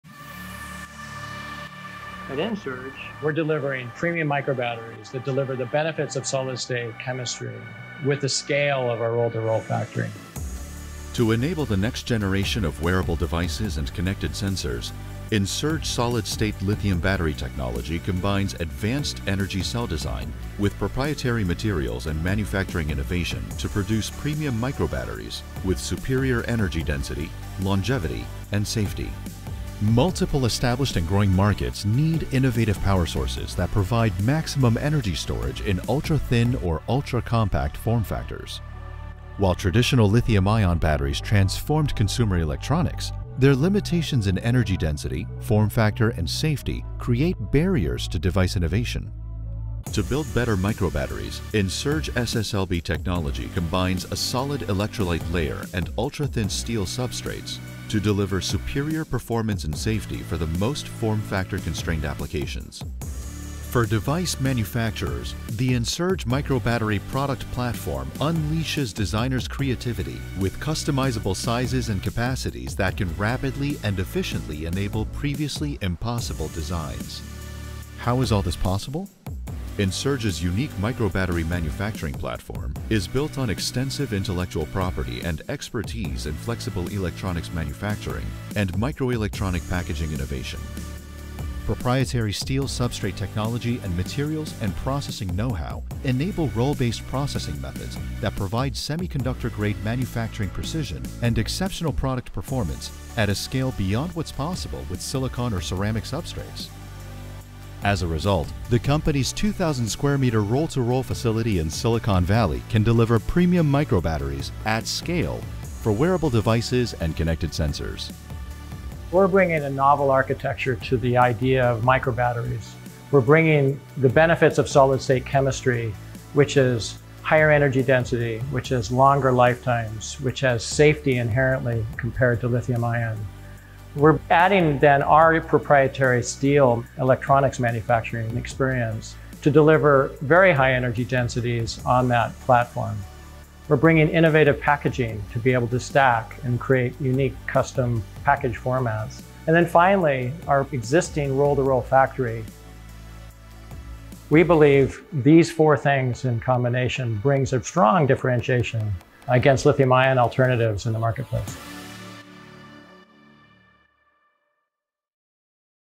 Ensurge Solid-State Lithium Rechargeable Microbattery
Ensurge is developing premium microbatteries combining the advantages of solid-state lithium battery (SSLB) technology with the scale of roll-to-roll manufacturing based on ultrathin steel substrates. With superior energy density, longevity, and safety compared to common lithium-ion alternatives, Ensurge SSLB microbatteries can enable the next generation of wearable devices and connected sensors.